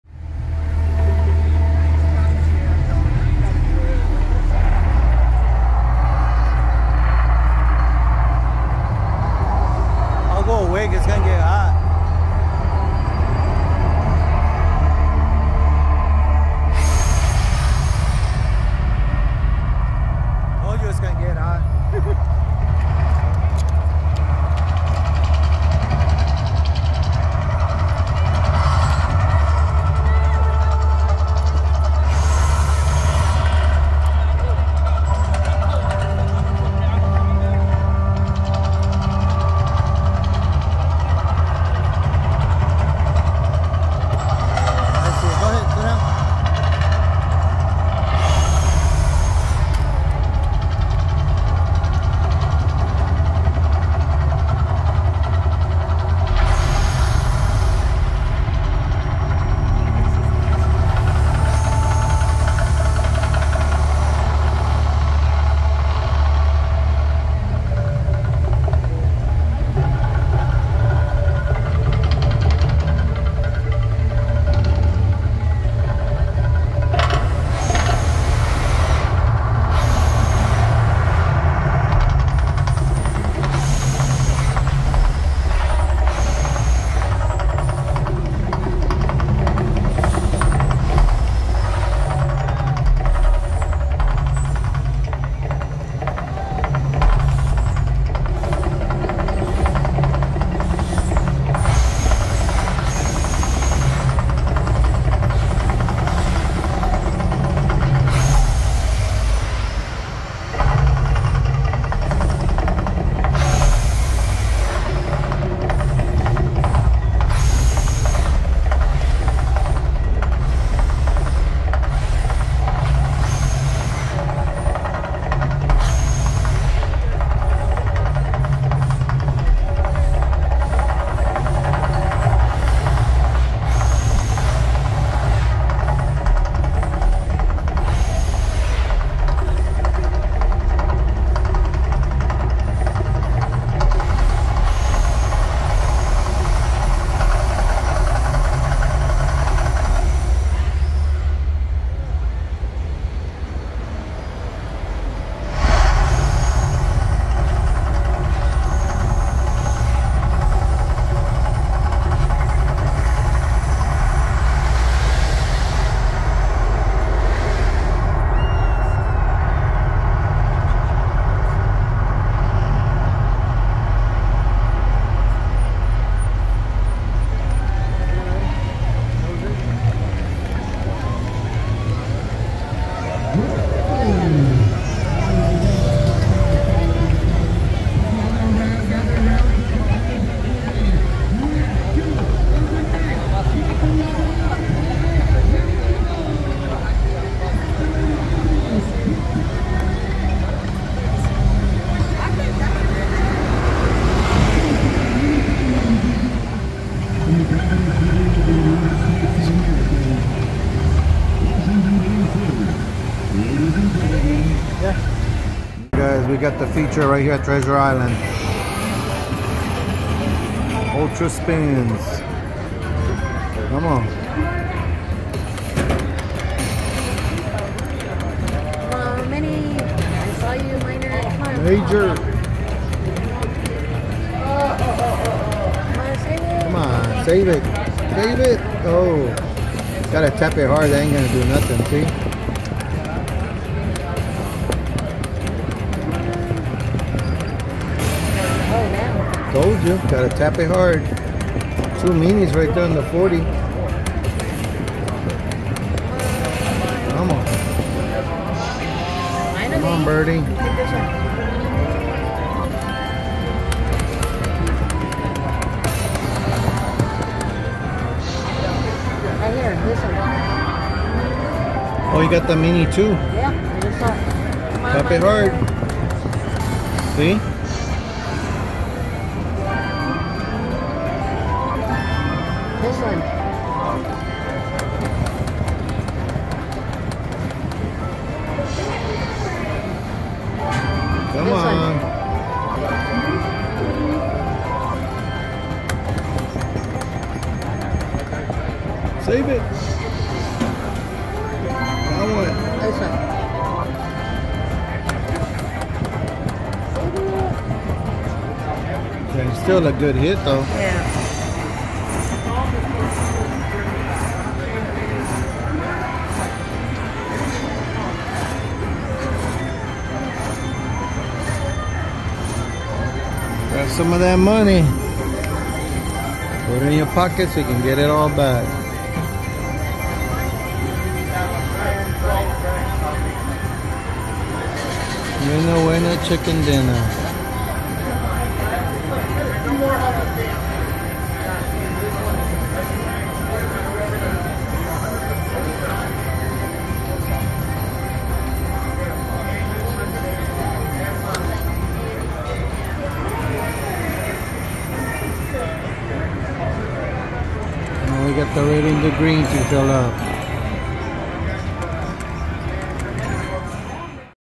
THE MIRAGE CASINO & RESORT MAYBE OUR LAST FIRE SHOW
DECIDED TO STICK AROUND FOR THE SHOW AND SOME SLOT PLAY